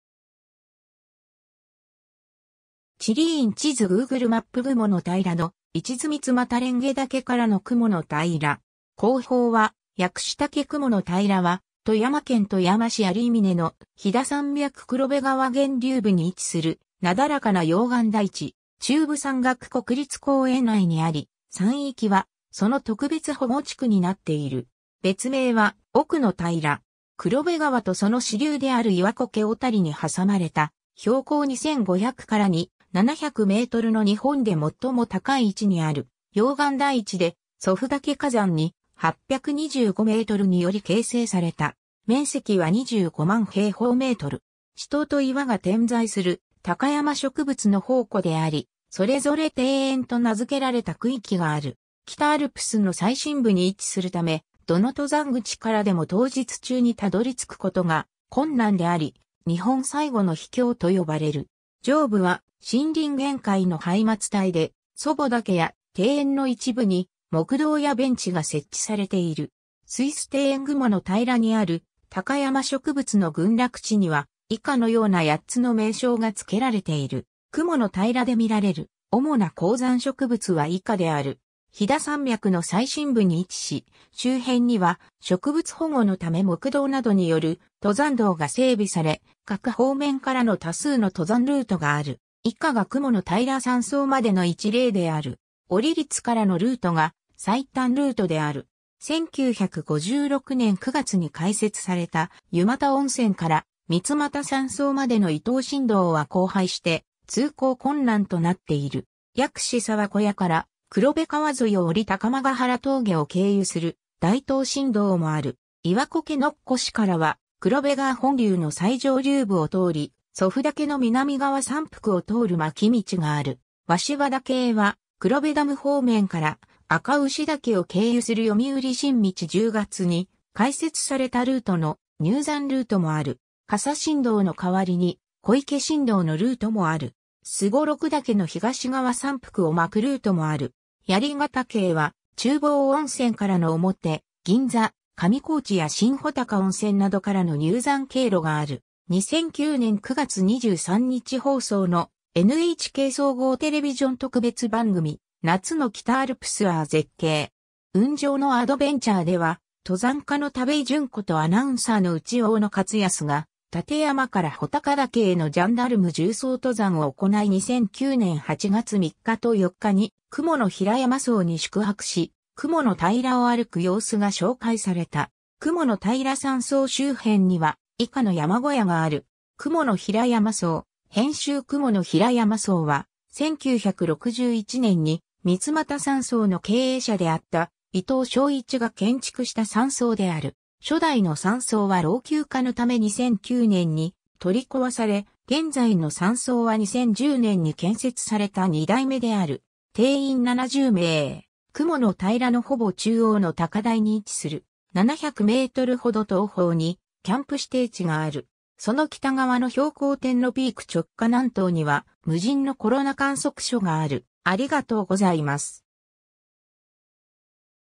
雲ノ平
地理院地図 Googleマップ 雲ノ平の位置図 三俣蓮華岳からの雲ノ平。
後方は薬師岳 雲ノ平（くものたいら）は、富山県富山市有峰の飛騨山脈（北アルプス）黒部川源流部に位置するなだらかな溶岩台地。
中部山岳国立公園内にあり、山域はその特別保護地区になっている。
別名は、奥ノ平。
 黒部川とその支流である岩苔小谷に挟まれた標高2,500 - 2,700 mの日本で最も高い位置にある溶岩台地で、祖父岳火山 2,825 m) により形成された。
面積は25万平方メートル。
池塘と岩が点在する高山植物の宝庫であり、それぞれ○○庭園と名付けられた区域がある。
北アルプスの最深部に位置するため、どの登山口からでも当日中にたどり着くことが困難であり、「日本最後の秘境」と呼ばれる。
上部は森林限界のハイマツ帯で、祖母岳や庭園の一部に木道やベンチが設置されている。
 スイス庭園 雲ノ平にある高山植物の群落地には、以下のような8つの名称が付けられている。
 雲ノ平で見られる主な高山植物は以下である。
 飛騨山脈の最深部に位置し、周辺には植物保護のため木道などによる登山道が整備され、各方面からの多数の登山ルートがある。
以下が雲ノ平山荘までの一例である。
折立からのルートが最短ルートである。
1956年9月に開設された湯俣温泉から三俣山荘までの伊藤新道は荒廃して通行困難となっている。
 ※薬師沢小屋から黒部川沿いを下り高天原峠を経由する大東新道もある。
 ※岩苔乗越からは、黒部川本流の最上流部を通り、祖父岳の南側山腹を通る巻道がある。
鷲羽岳へは、黒部ダム方面から赤牛岳を経由する読売新道（読売新聞の事業として、1961年（昭和36年）10月に開設されたルート）の入山ルートもある。
 ※笠新道の代わりに小池新道のルートもある。
双六岳の東側山腹を巻くルートもある。
 槍ヶ岳へは、中房温泉からの表銀座、上高地や新穂高温泉などからの入山経路がある。
 2009年9月23日放送のNHK総合テレビジョン特別番組『夏の北アルプス あぁ絶景！雲上のアドベンチャー』では、登山家の田部井淳子とアナウンサーの内多勝康が立山から穂高岳へのジャンダルム縦走登山を行い2009年8月3日と4日に雲ノ平山荘に宿泊し、雲ノ平を歩く様子が紹介された。
 雲ノ平山荘 周辺には以下の山小屋がある。
 雲ノ平山荘[編集] 雲ノ平山荘は、1961年に三俣山荘の経営者であった伊藤正一が建築した山荘である。
初代の山荘は老朽化のため2009年に取り壊され、現在の山荘は2010年に建設された二代目である。
定員70名。
雲ノ平のほぼ中央の高台に位置する。
700 mほど東方に、キャンプ指定地（テント約50張り、給水施設あり）がある。
その北側の標高点 (2,576 m) のピーク直下南東には無人のコロナ観測所がある。